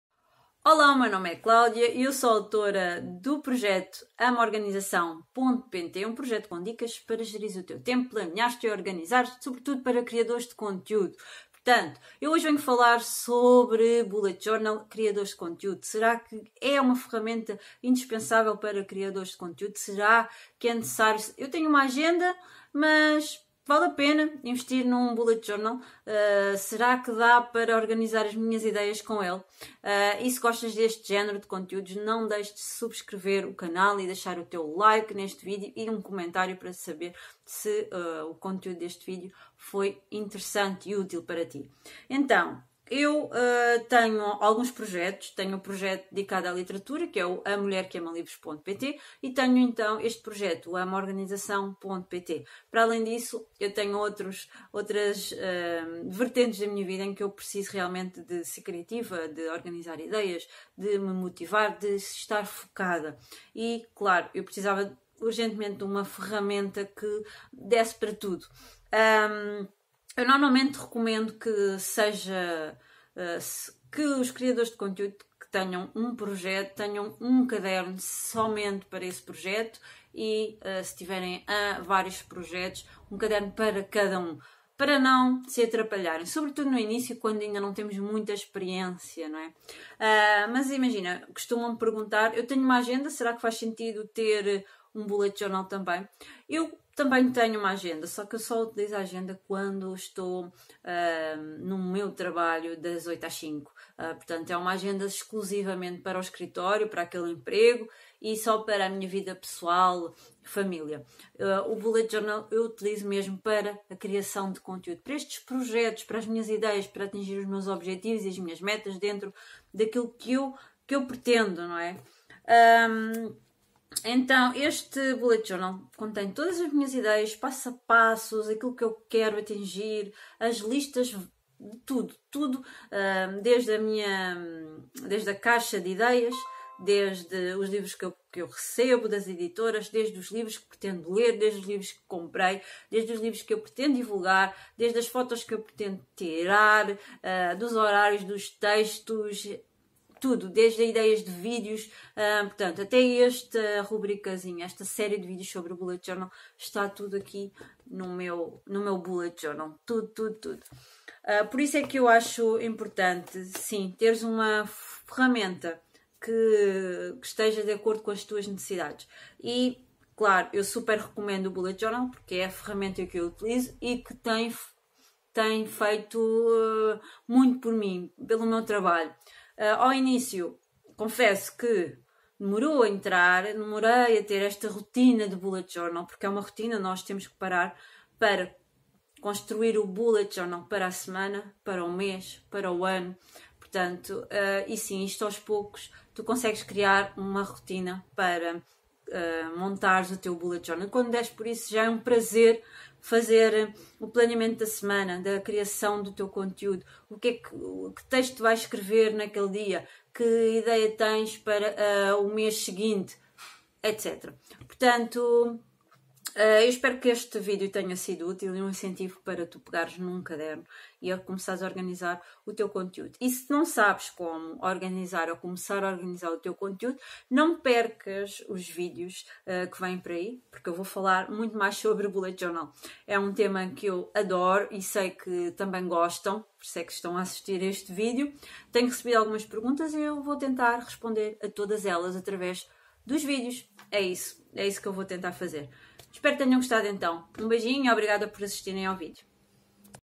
ORGANIZAR CONTEÚDO E IDEIAS COM UM BULLET JOURNAL
Será que é necessário ter um Bullet Journal para organizares as tuas ideias?